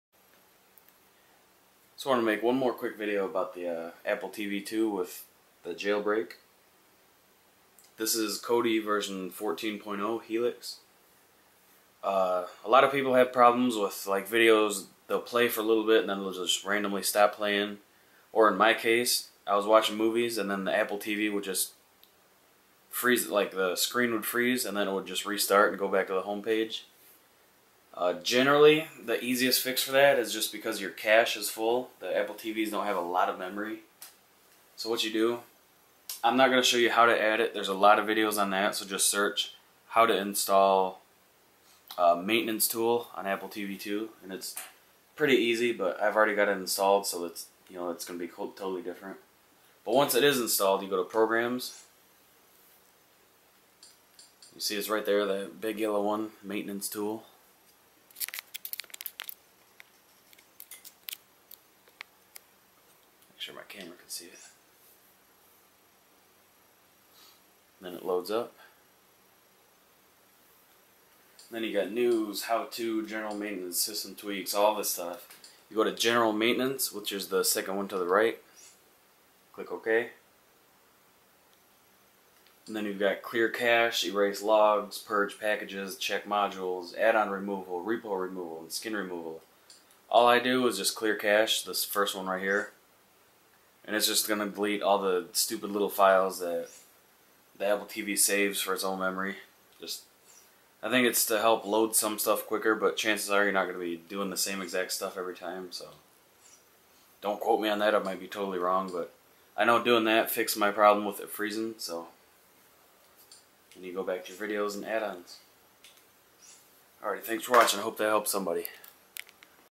How to use Maintenance Tool on Kodi (Apple TV 2)
Are you having problems with videos buffering alot, or freezing and going back to the main menu or Kodi?

Chances are, your Cache needs to be cleaned out. This tool on Kodi will help you.

Chances are you also dont have this installed yet, so search "How to add maintenance tool to Apple TV 2" and you should find plenty of how-to videos!